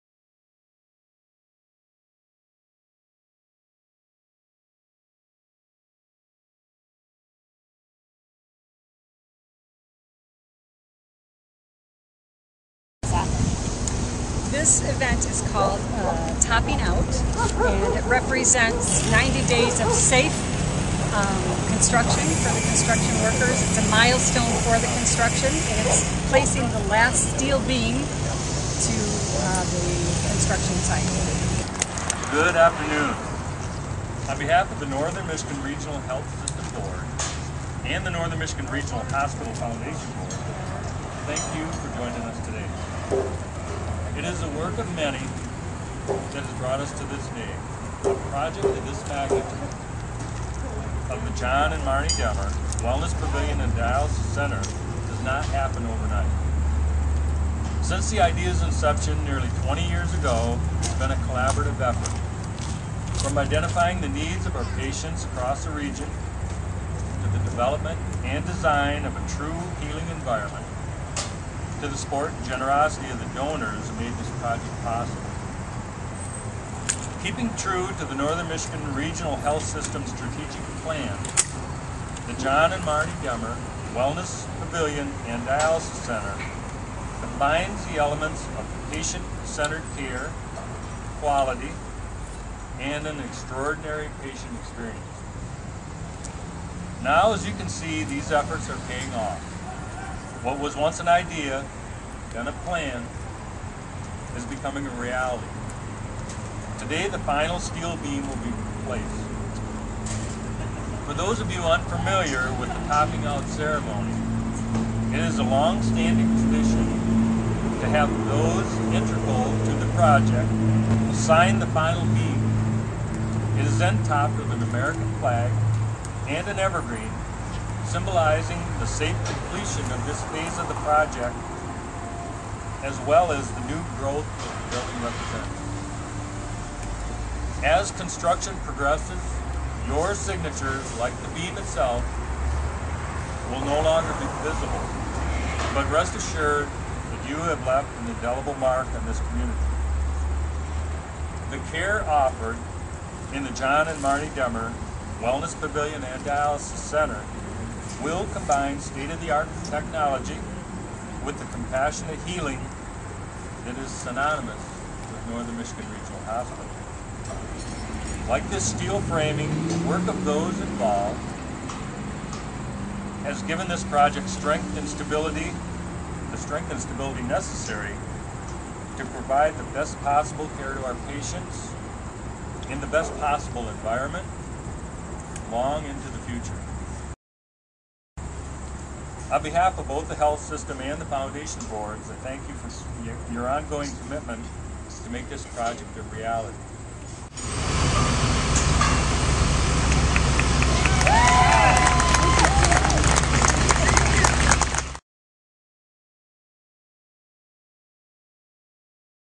John and Marnie Wellness Pavilion and Dialysis Center
On Monday, Aug. 23, hospital representatives, donors and local officials signed the last steel beam of the new John and Marnie Wellness Pavilion and Dialysis Center, which is located at the Northern Michigan Regional Hospital's Lockwood-MacDonald campus in Petoskey.